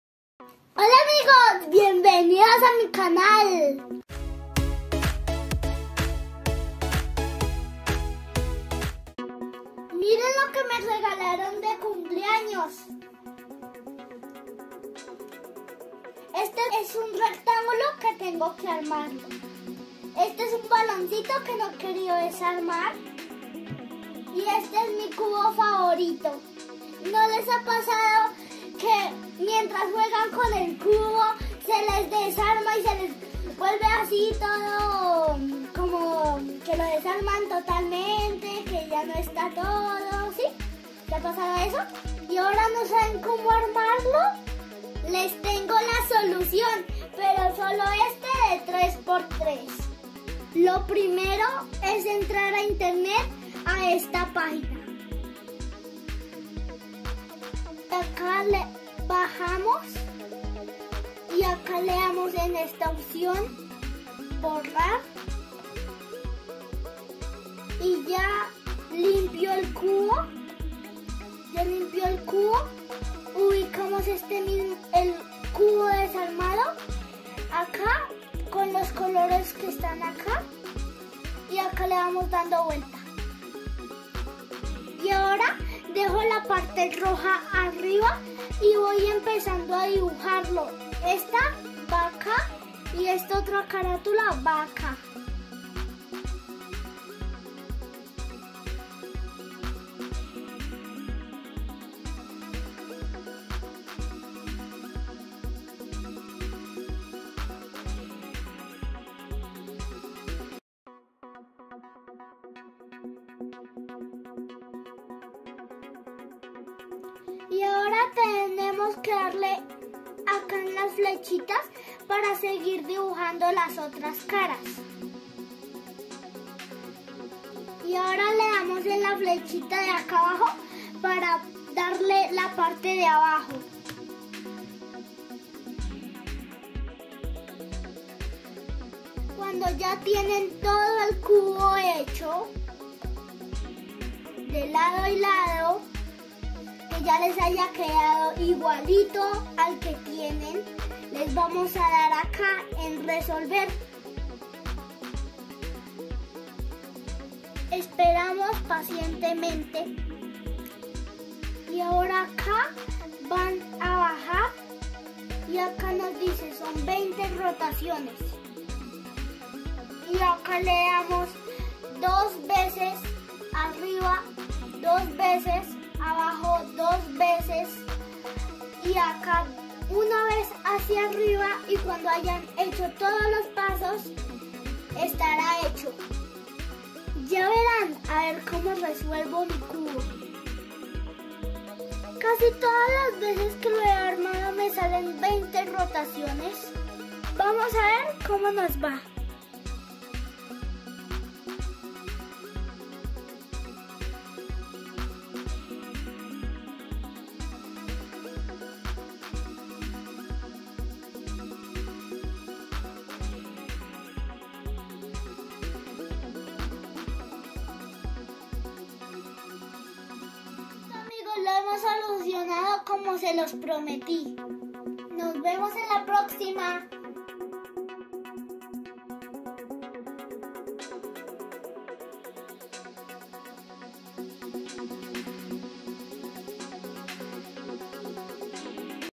Encontré una página que me ayuda a resolver el Cubo de Rubik de 3x3!
Hola Creador@s!
Te explicamos paso a paso como armar completamente tu cubo rubik de 3x3, vamos a verlo!

COMENTA AQUÍ ABAJO...